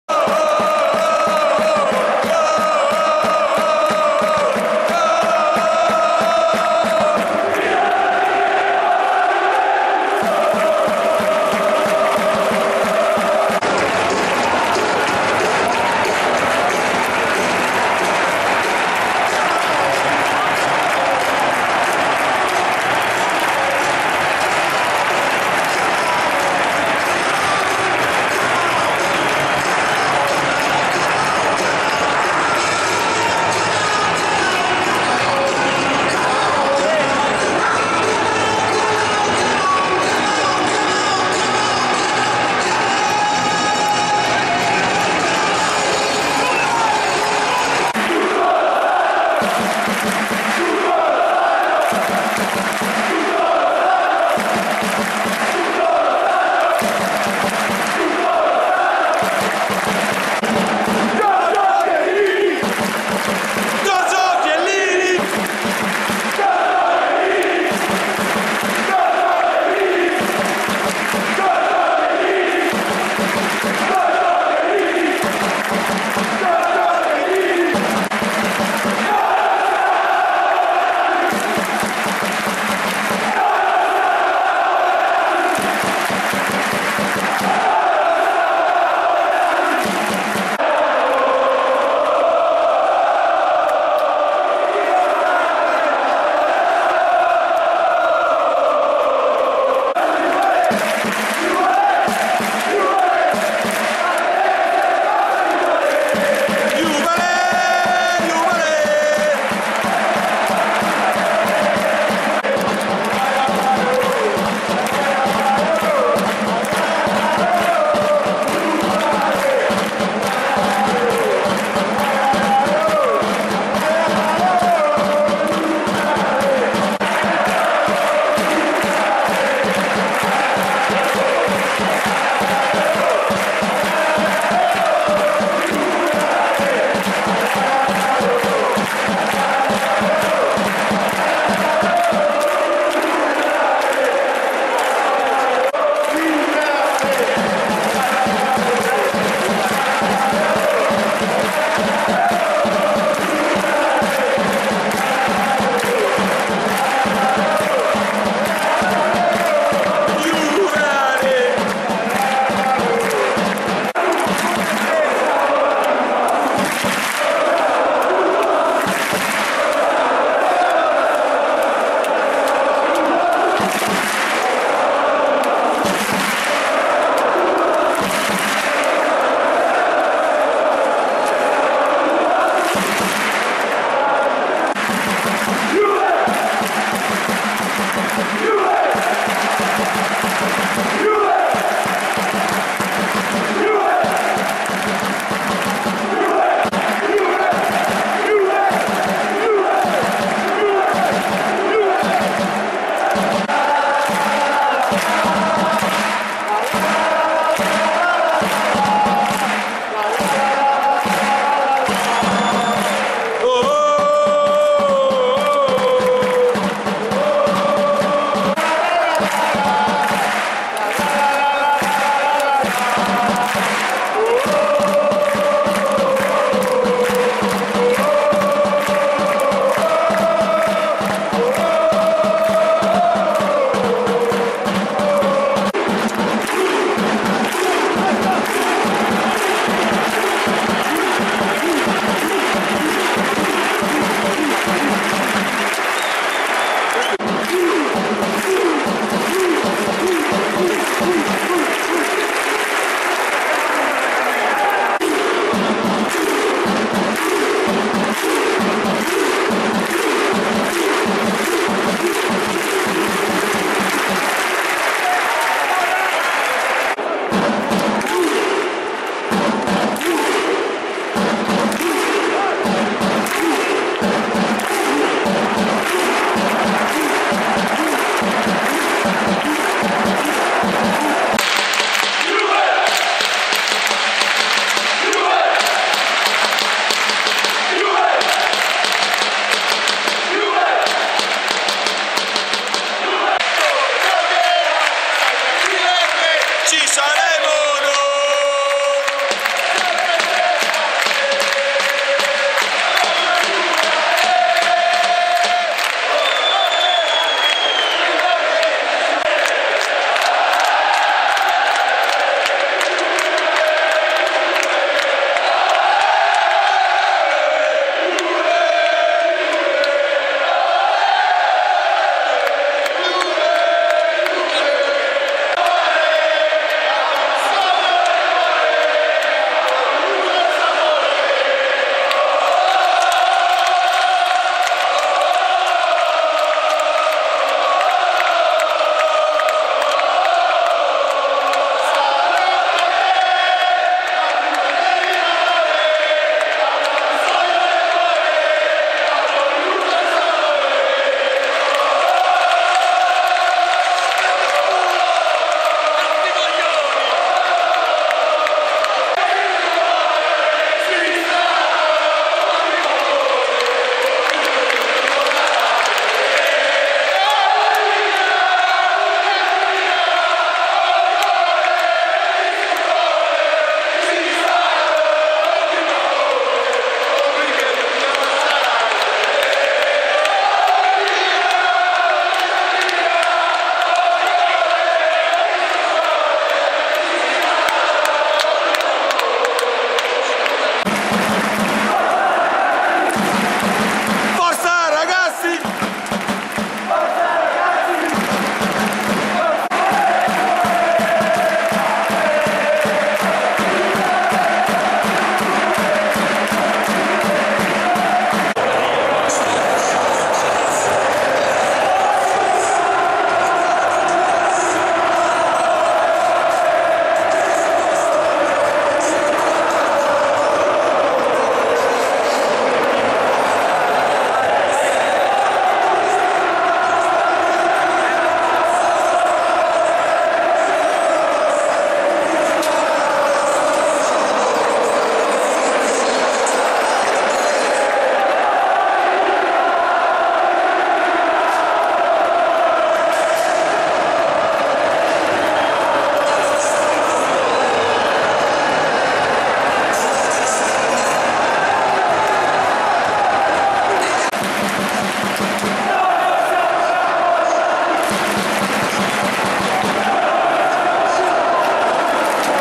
JUVENTUS Vs Napoli CURVA SUD
Serie A 2^ giornata  JUVENTUS Stadium Torino 31/08/2019
4--3(Danilo,Higuaín,Cristiano,Manolas,Lozano,Di Lorenzo,Aut.Koulibaly)